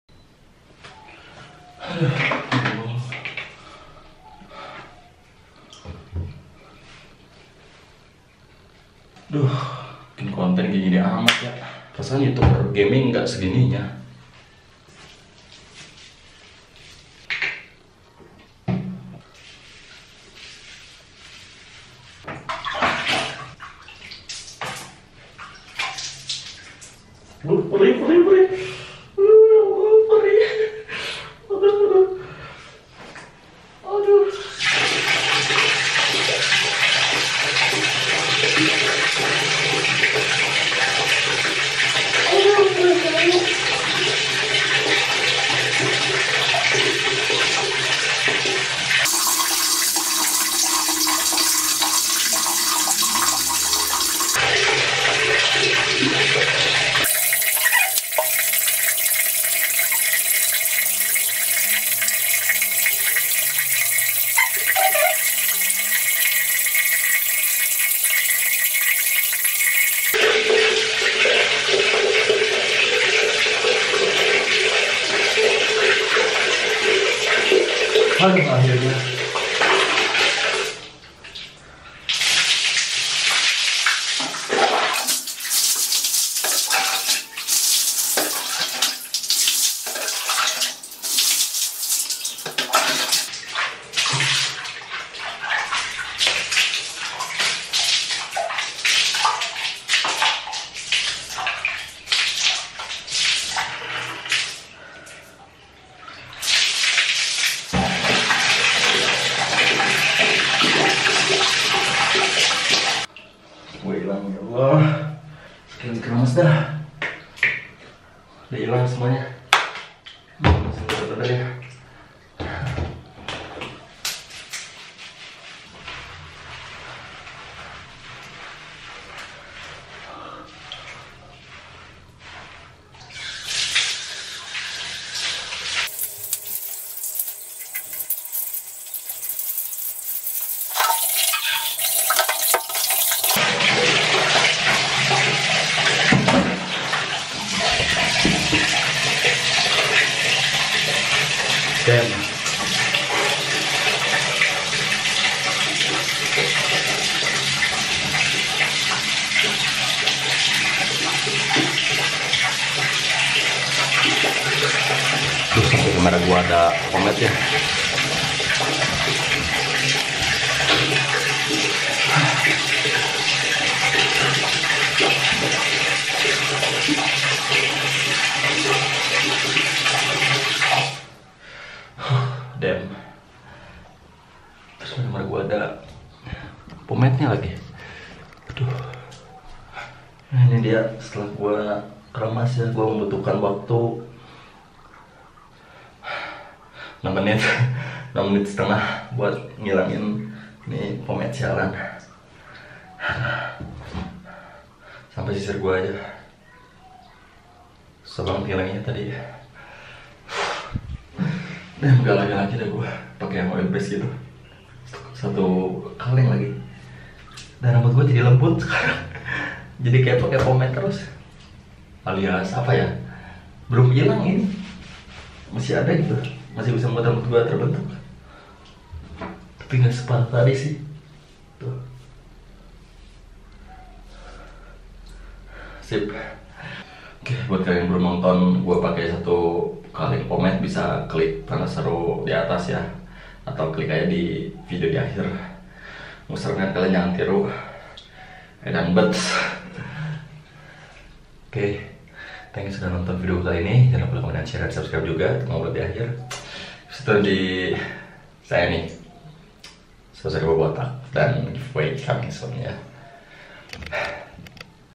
KERAMAS TERLAMA! BERSIHIN SATU KALENG POMADE DI KEPALA (POMADE SIALAN!)
Ingat video ini?

Jadi setelah gua pake satu kaleng pomade di kepala, pada episode ini gua ngedegrease alias keramasin rambut gua biar pomadenya ilang. Hasilnya apa? KOCAGGG!!! Langsung tonton aja yak!